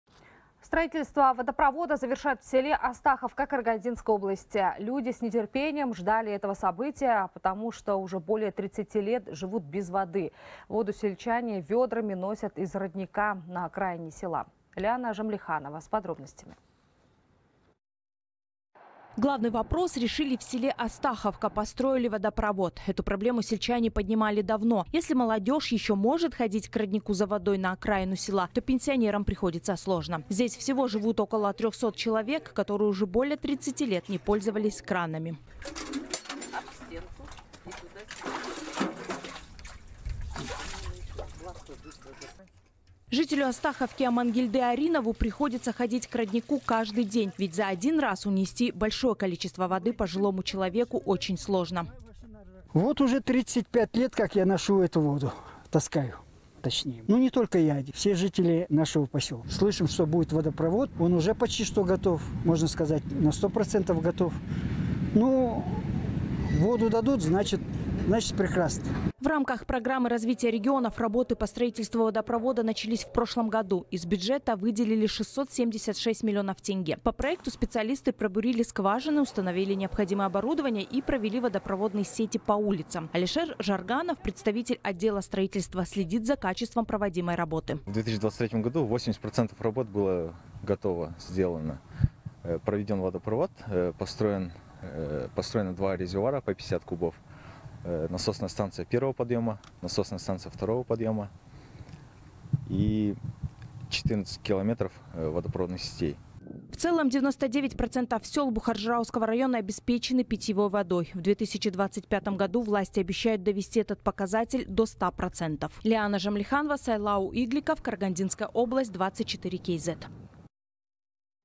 30 лет ждали водопровода в селе Карагандинской области
Строительство водопровода завершают в селе Астаховка Карагандинской области, сообщает «24KZ». Люди с нетерпением ждали этого события, потому что уже более 30 лет живут без воды. Ее сельчане вёдрами носят из родника на окраине села.